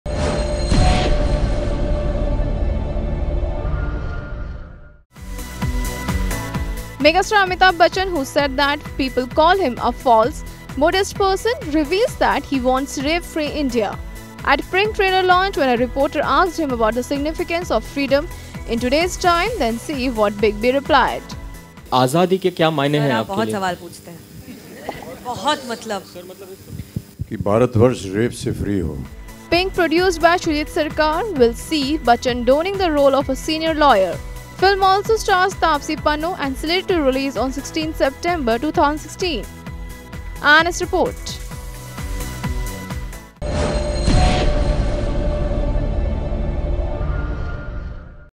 I want RAPE FREE India: Amitabh Bachchan
Megastar Amitabh Bachchan who said that people call him a false modest person, reveals that he wants rape free India. At 'PINK' trailer launch, when a reporter asked him about the significance of freedom in today’s time then see what Big B replied: